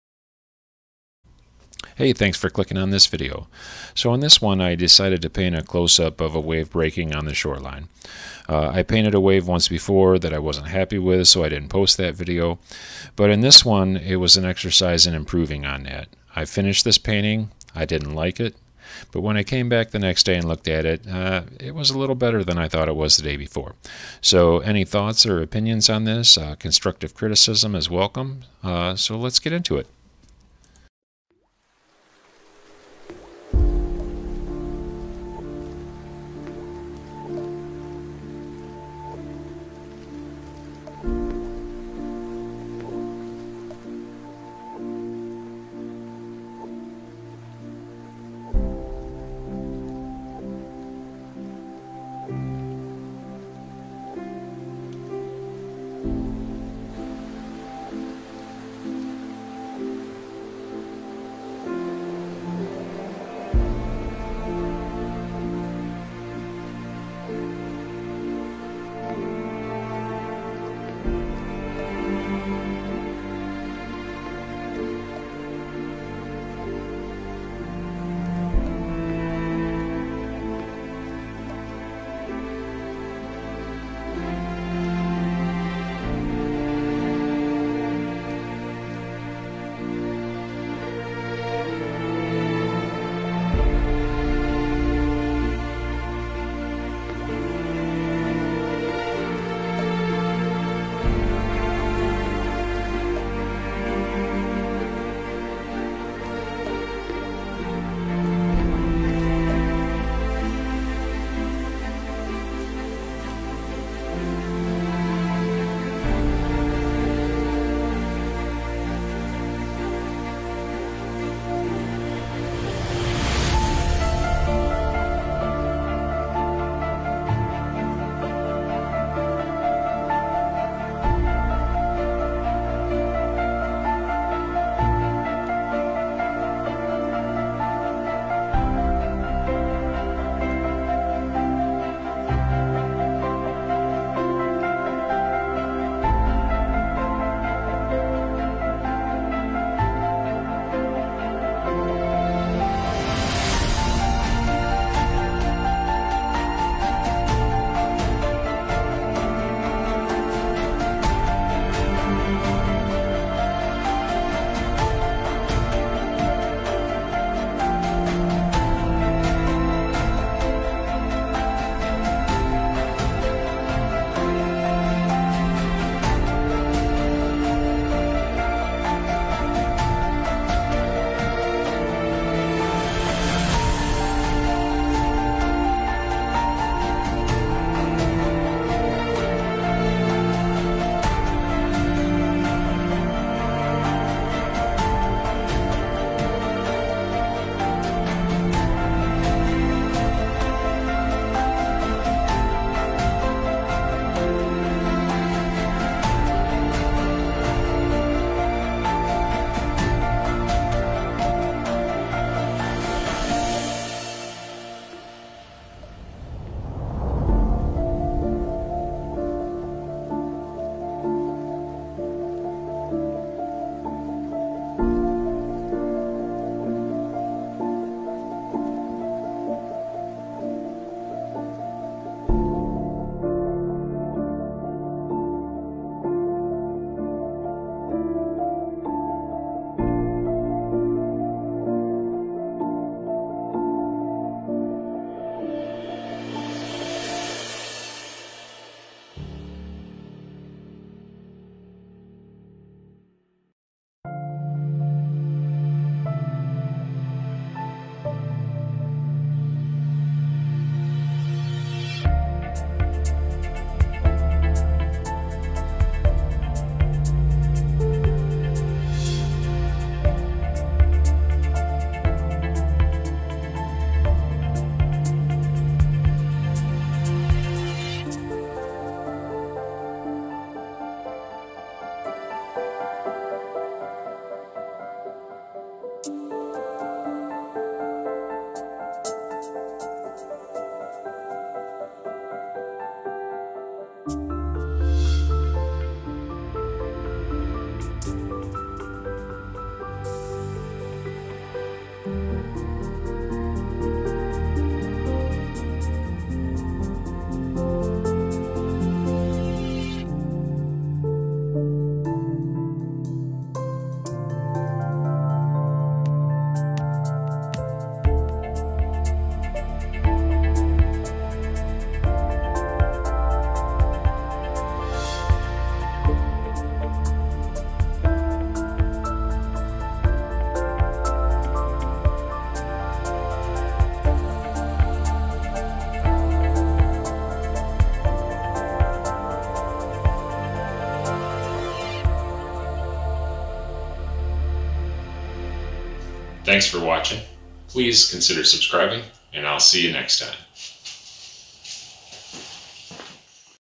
Painting Breaking Waves in Acrylic
#87 I live in Michigan and this would be fairly common scene if you go to Lake Michigan or Lake Huron shorelines. I feel like I still need to work on my wave technique but I'm slowly getting better at it with practice.